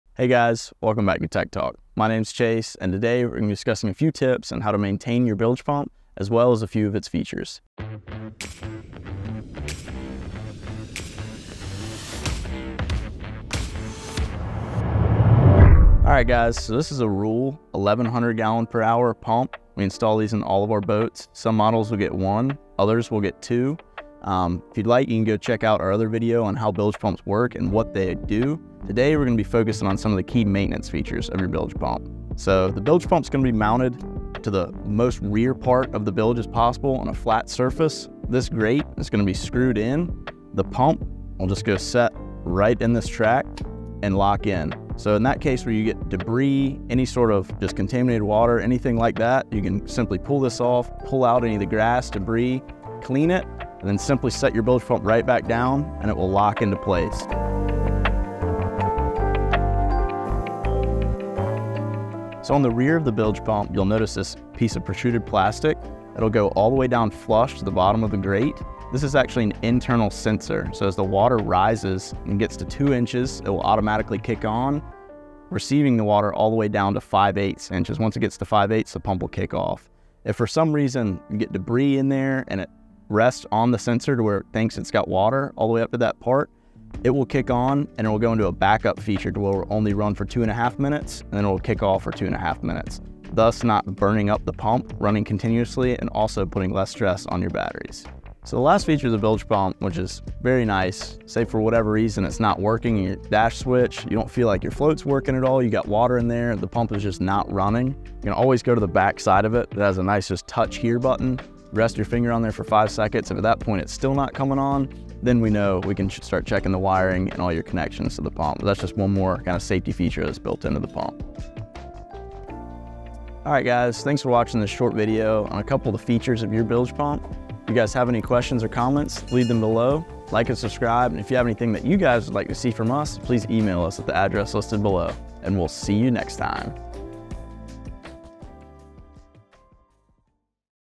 Tech Talk - Mastering Bilge Pump Maintenance - Essential Boating Safety
In this episode of Tech Talk, Chase walks you through a few simple steps on how to maintain your bilge pump. These simple steps can help keep your bilge pump from failing and leading to a dangerous situation.

0:00 Intro
0:16 Rule Pump
1:02 Internal Censor
1:38 Safety Feature
2:03 Outro